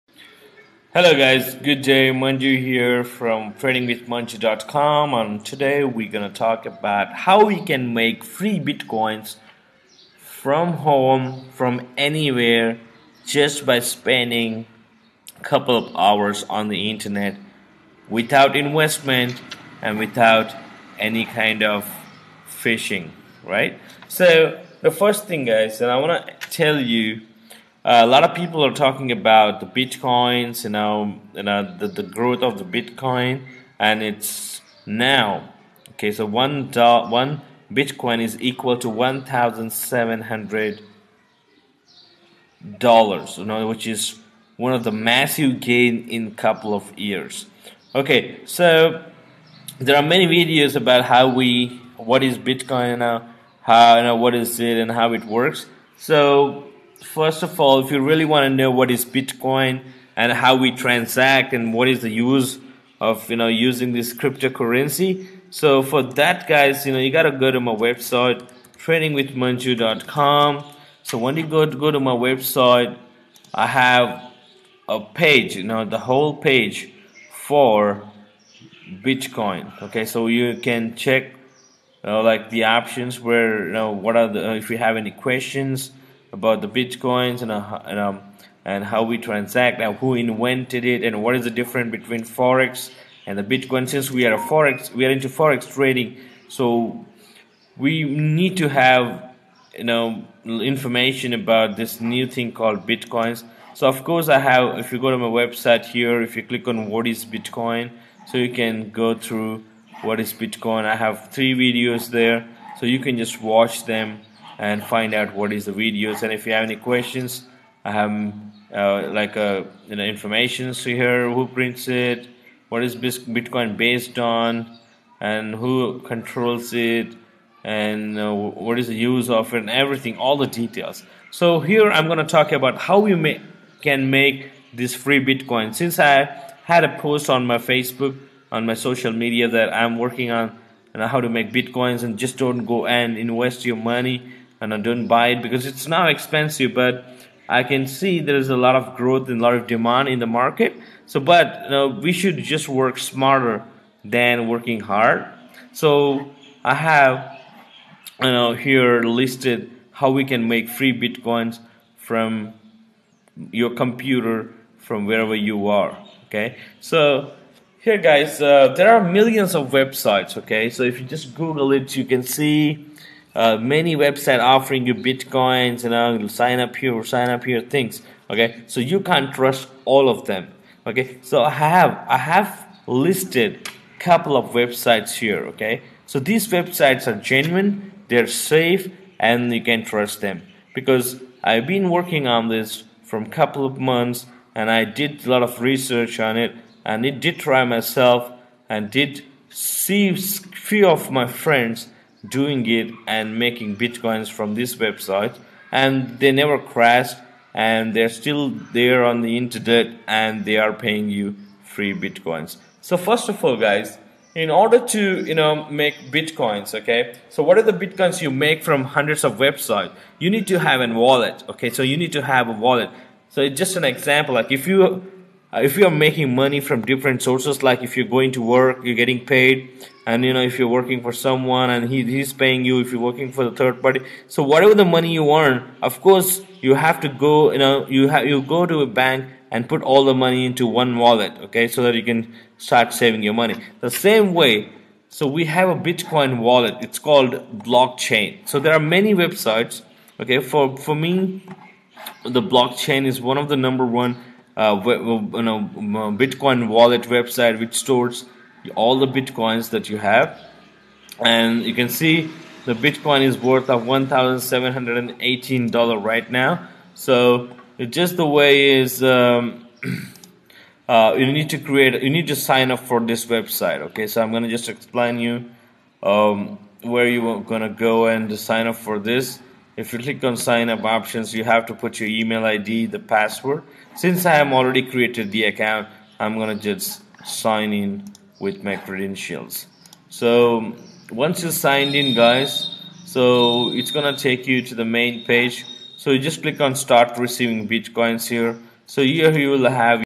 How to make free Bitcoins? working from home withtout investment !?
Video that explains how to make free Bitcoins from home or anywhere by just spending 2 hours a day without investing single penny.